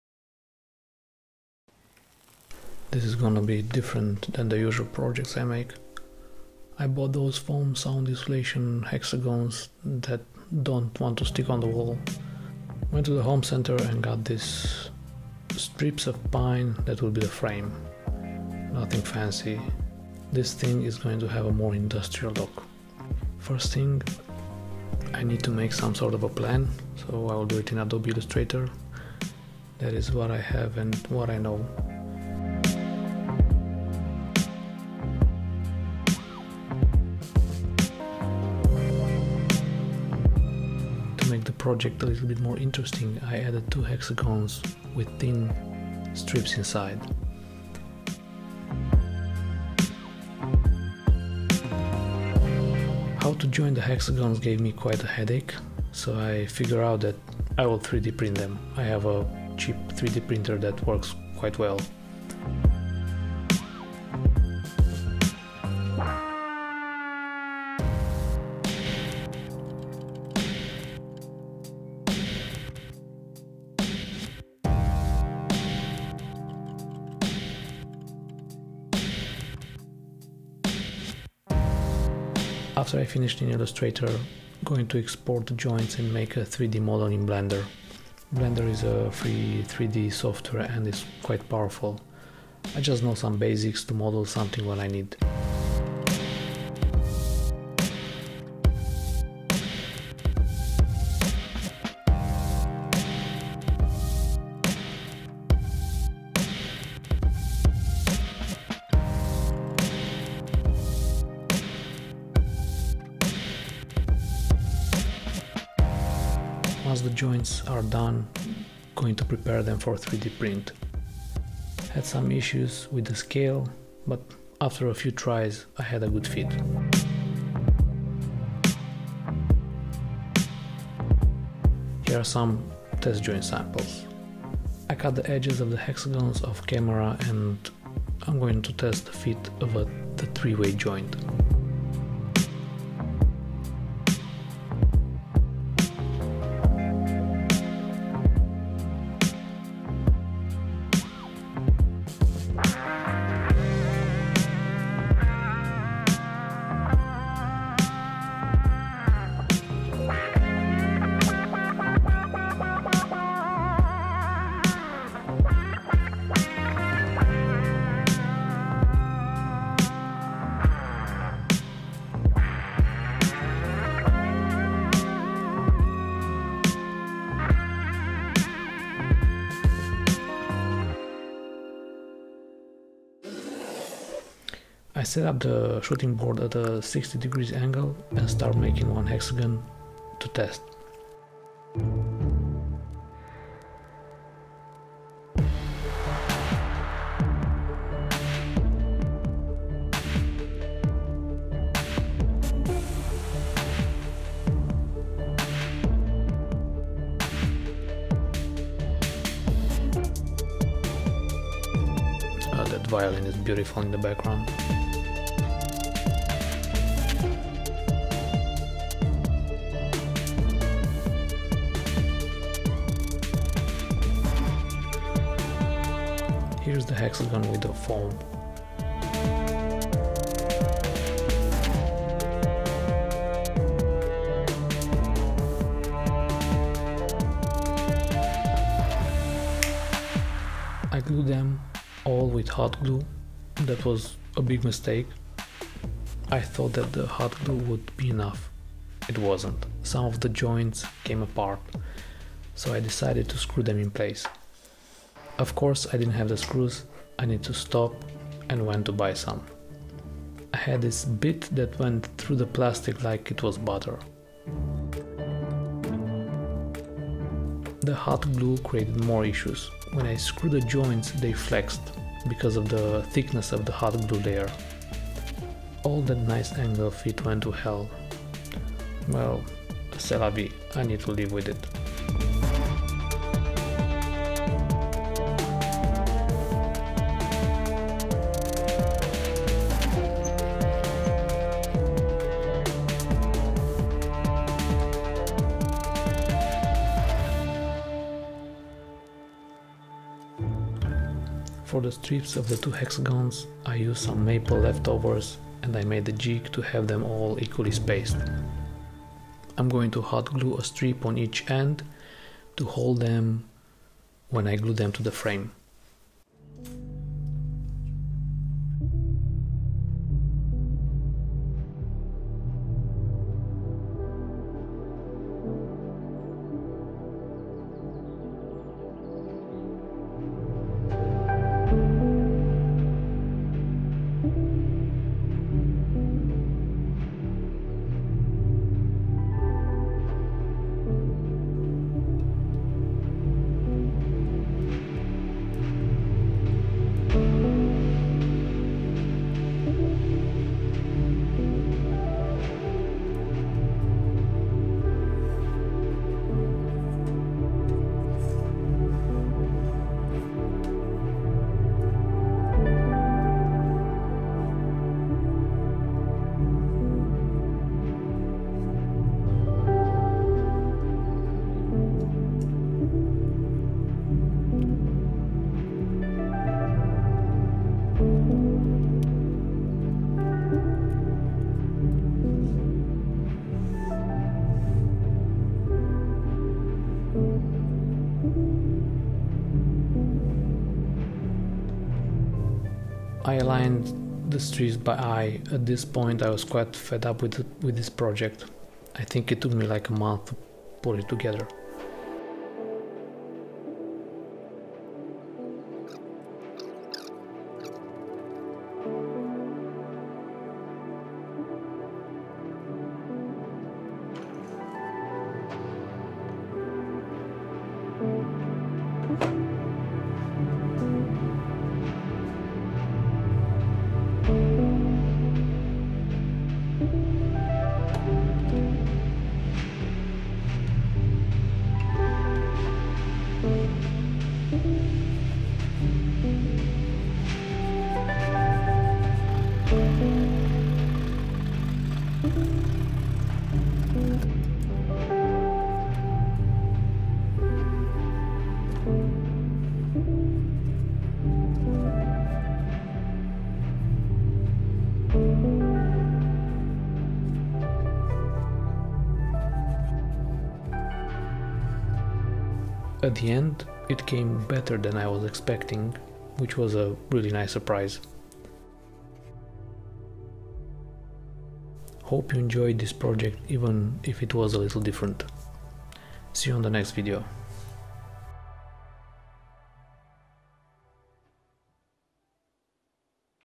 Honeycomb frame with 3d printed joints for sound foam insulation
I bought these sound foam hexagons to take out some of the echoes, but I can`t make them stick to the wall. So I decided to make a wood frame from some cheap pine strips and 3d print the joints.